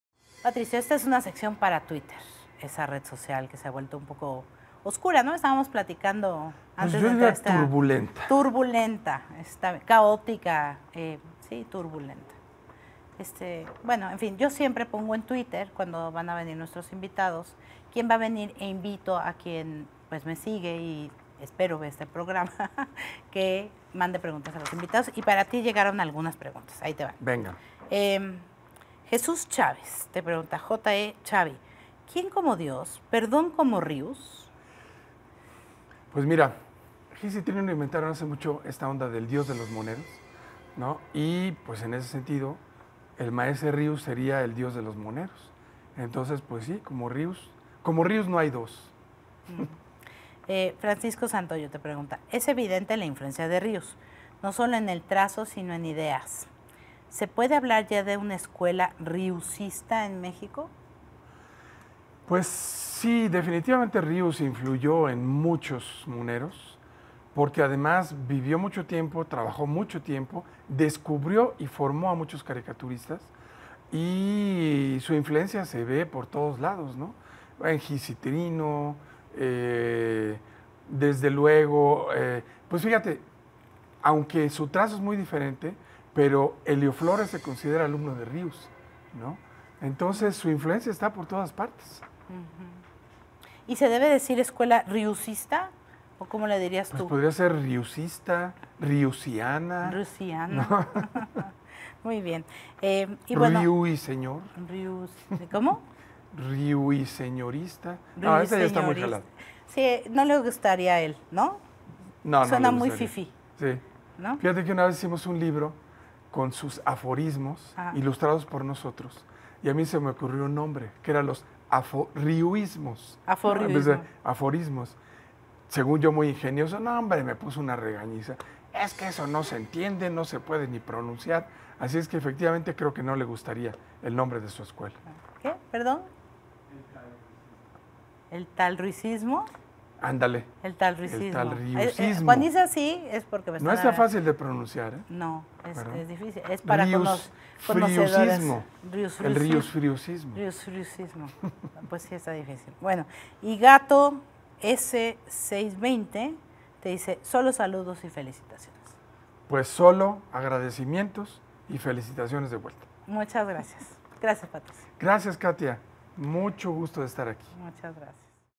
Tus preguntas a Patricio Ortiz González
Katia D’Artigues recibe esta semana a Patricio Ortiz González, conocido popularmente como "Patricio Monero", para platicar acerca de su libro "México antes de ser México", primero de una serie de varios tomos en los que, al estilo de las historietas de 'Rius', nos cuenta acerca de las civilizaciones más antiguas que habitaron lo que hoy es el territorio nacional. El afamado caricaturista también nos revela algunos de sus siguientes proyectos y cómo distinguir un hecho de una idea distorsionada de la realidad, entre otros temas.